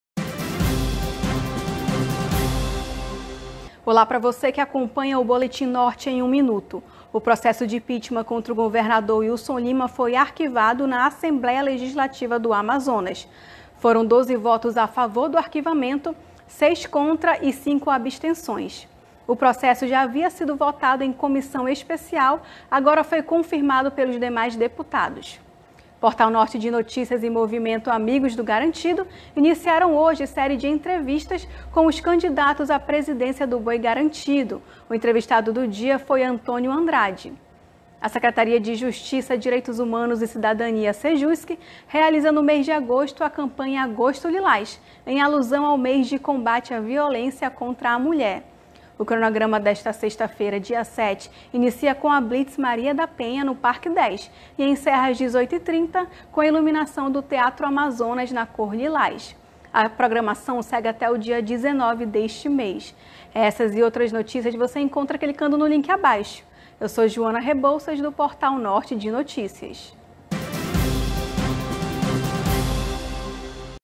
Boletim
Processo de impeachment contra o Governador Wilson Lima é arquivado.
Entrevista com candidatos a presidência do Boi Garantido.
Campanha Agosto lilás realizada pela Sejusc.